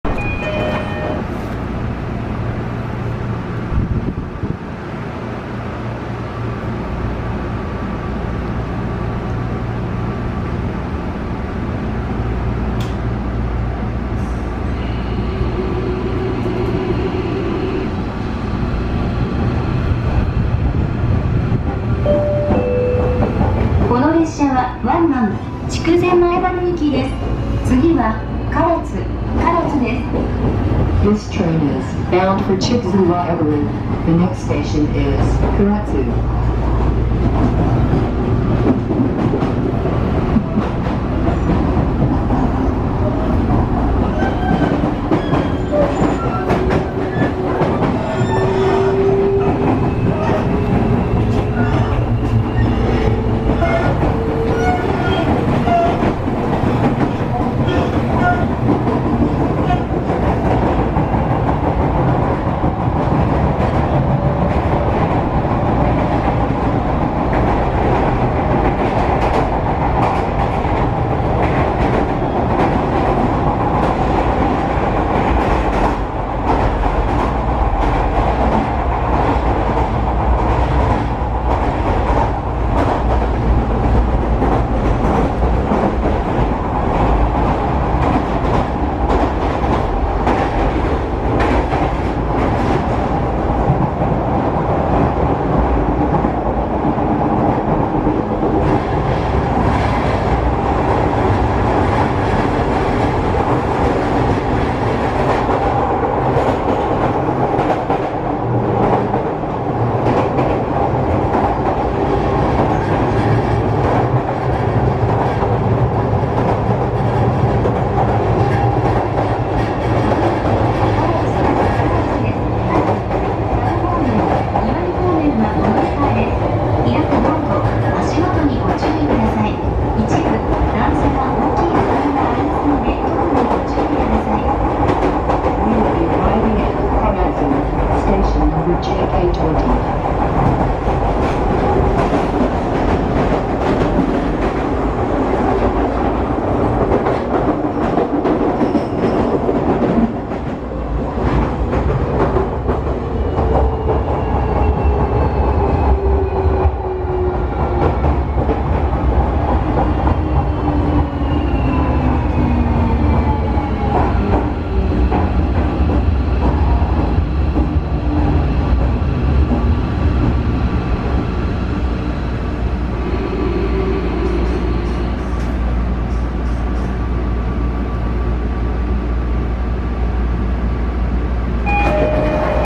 JR九州305系W02(モハ304-102)を1両貸切でした。
この日は唐津市で花火大会が開催されたため観客輸送を確保するため、所定の103系3両から305系6両に車両変更となりました。
最近、筑前前原～西唐津間でも6両ワンマン運転が開始されました。